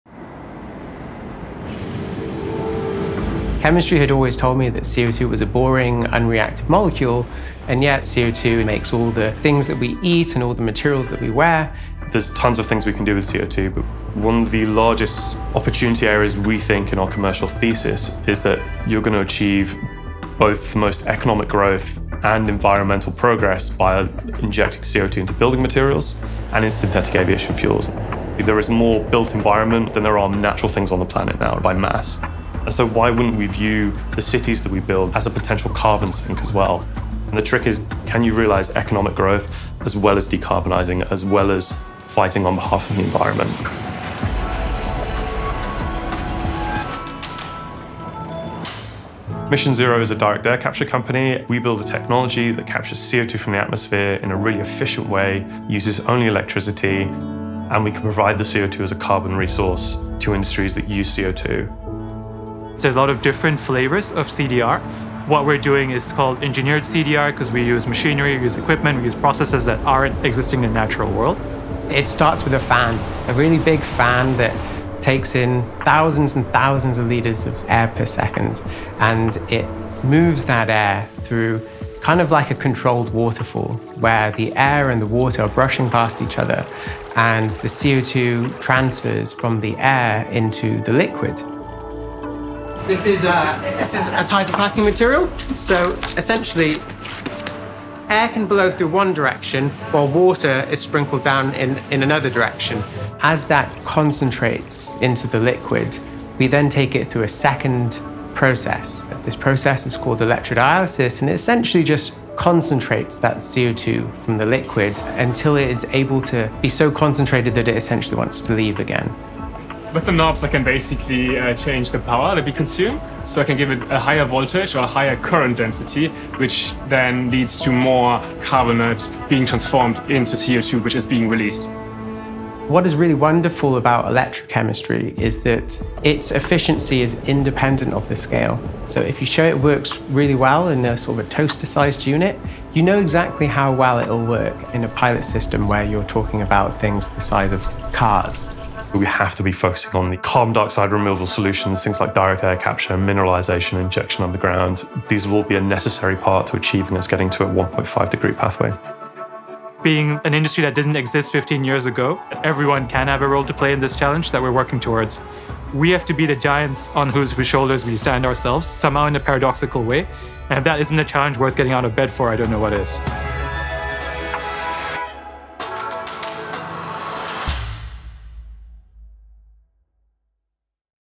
Frontier | Capturing CO₂ from the air
Mission Zero drew inspiration from the way the human body transports CO₂ from our tissues, to our lungs, and out through our breath to build a technology that captures CO₂ from the air.

“It starts with a fan, a really big fan that takes in thousands and thousands of liters of air per second and it moves that air through kind of like a controlled waterfall where the air and the water are brushing past each other and the CO₂ transfers from the air into the liquid.”

Frontier is an advance market commitment (AMC) that aims to accelerate the development of carbon removal technologies by guaranteeing future demand for solutions like the one Mission Zero is building. It’s funded by Stripe, Alphabet, Shopify, Meta, McKinsey, and tens of thousands of businesses using Stripe Climate. Climate models project that by 2050 we will need to permanently remove billions of tons of CO₂ from the atmosphere every year.